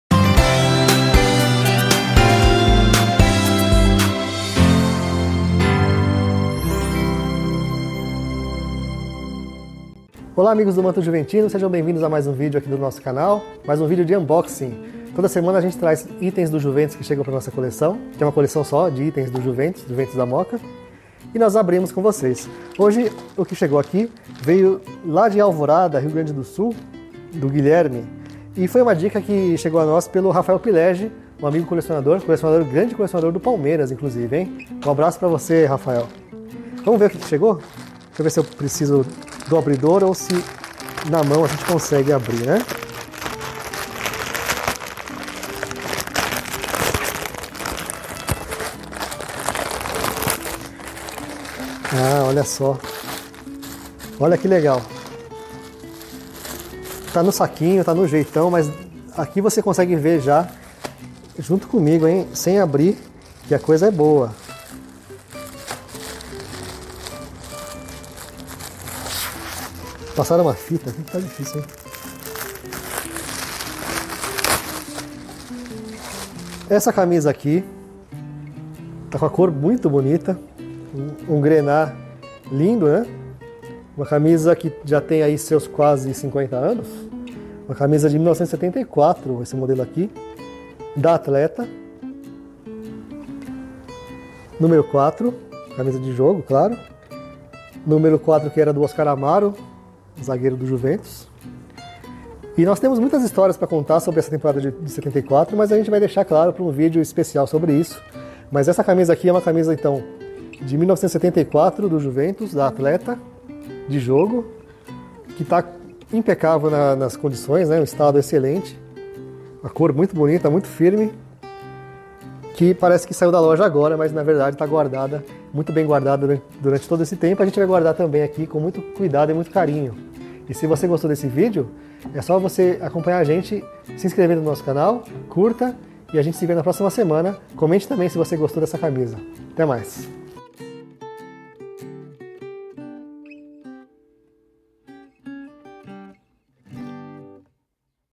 Recebemos uma camisa muito antiga Athleta do Juventus | Manto Juventino unboxing
Vamos ver mais um item que chegou à nossa coleção